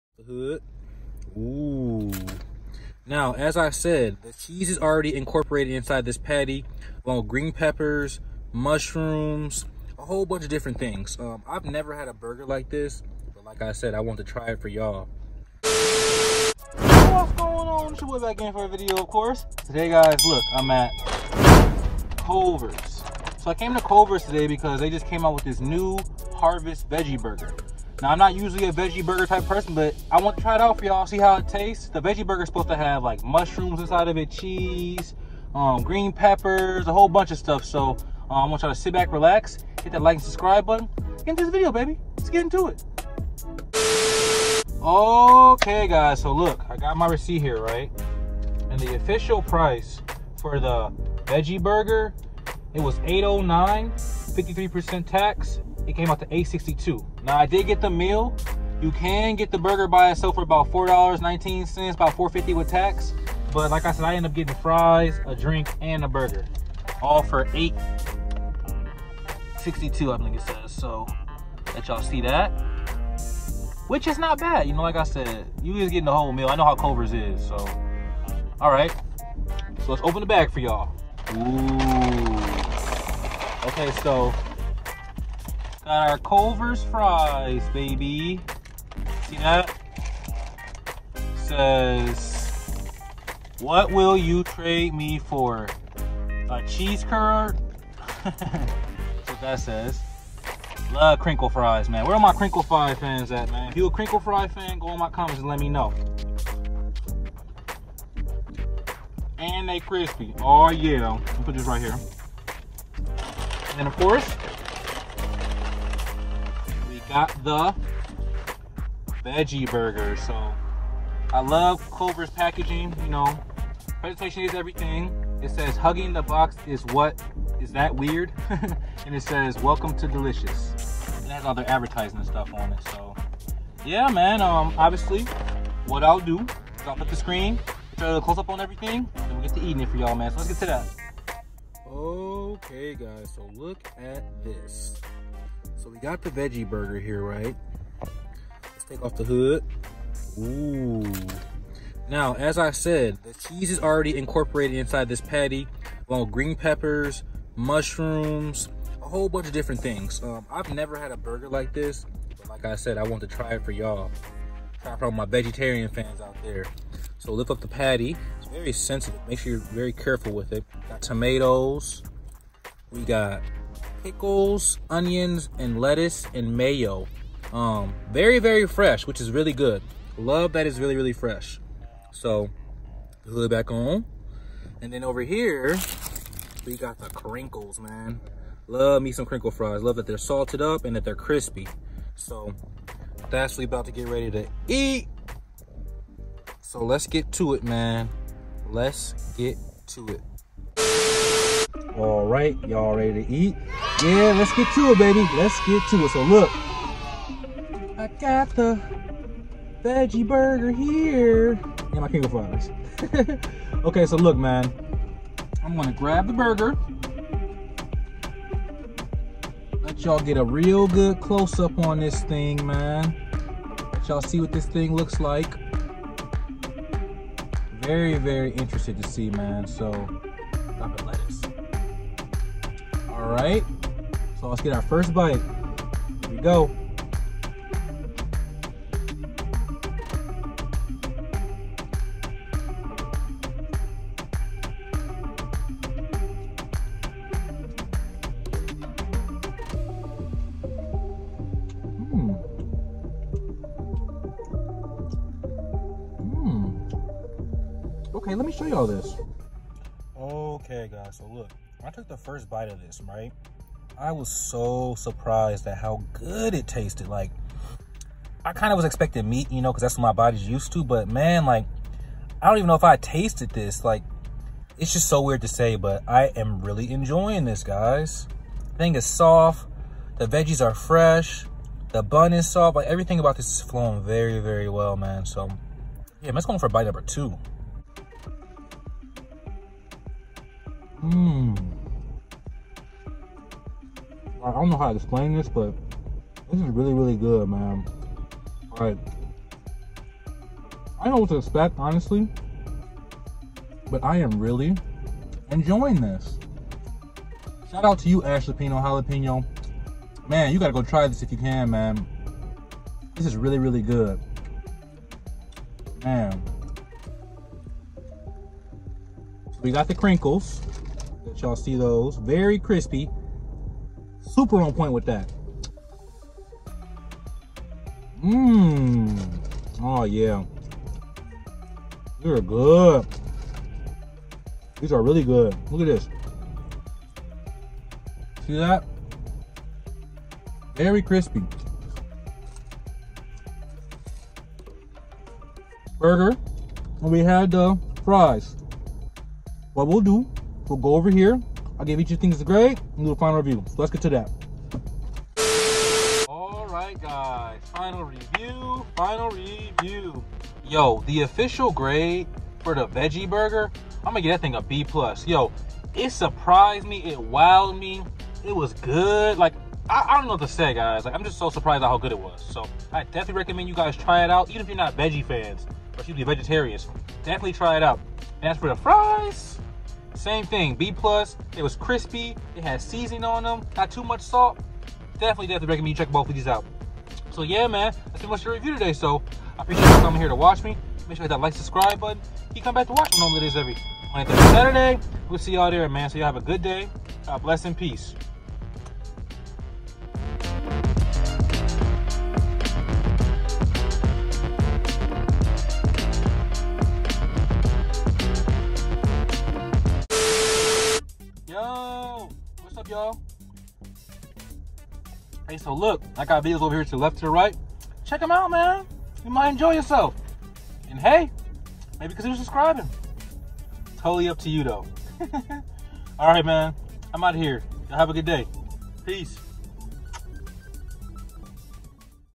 Culver's New Harvest Veggie Burger Review! (Beyond Meat)
Today I went to Culver's. I went there to do the Culver's NEW Harvest Veggie Burger Review. I am not usually a Veggie Burger fan but I wanted to try this for you guys. Come see my full reaction to this Veggie Burger. I hope you enjoy.

Any support is appreciated. God bless guys, and stay healthy.